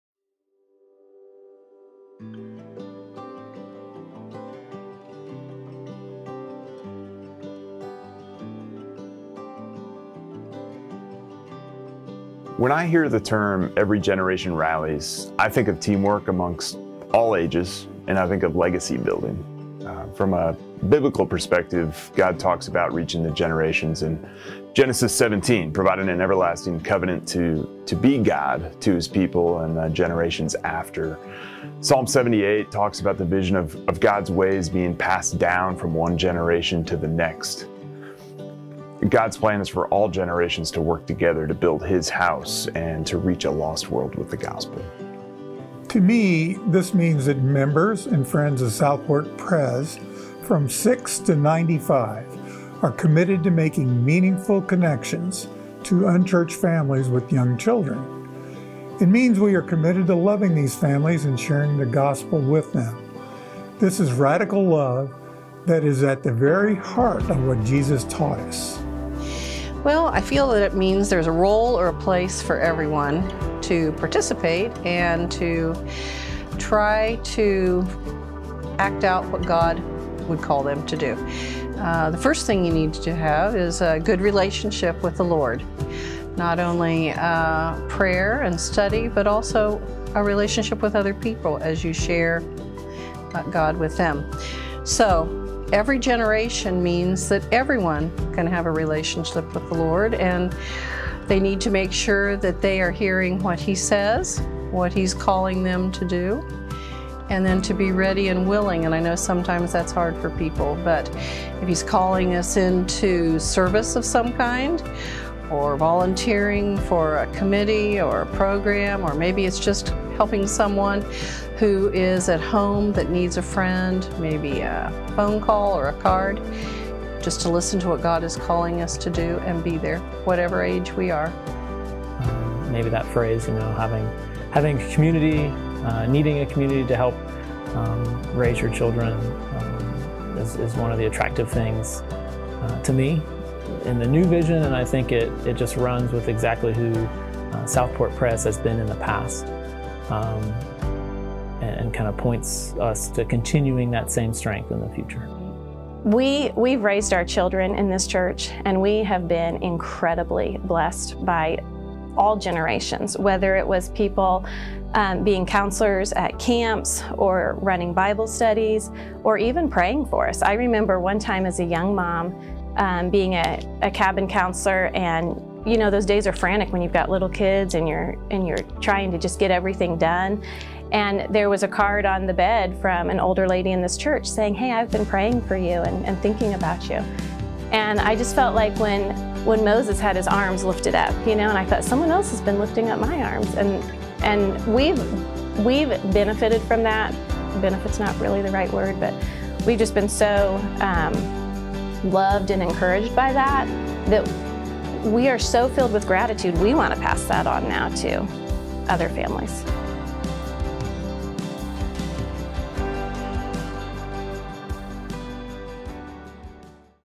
What does Every Generation Rallies mean?
We asked our elders this question: When you hear the phrase "Every Generation Rallies", what does that mean to you?

We want to rally our congregation together to serve young families in our community.